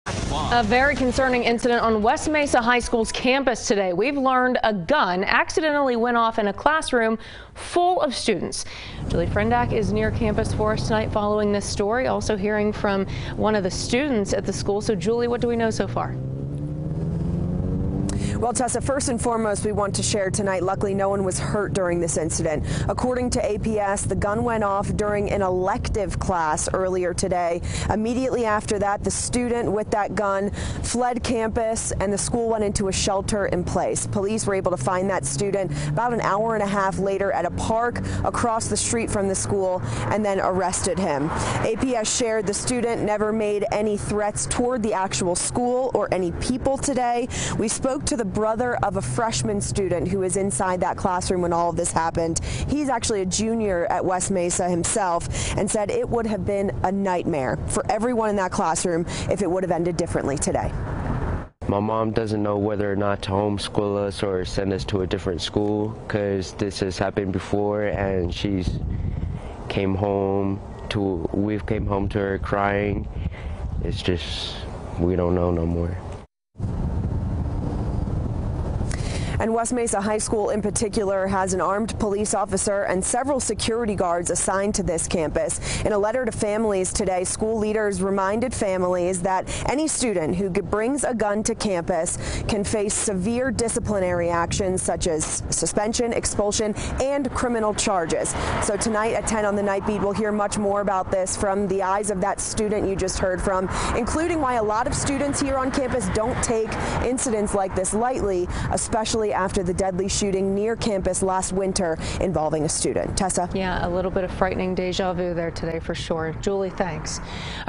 APS: West Mesa HS student arrested after gun accidentally goes off in classroom
A West Mesa High School student was arrested by police Wednesday after a gun accidentally went off in a classroom full of students.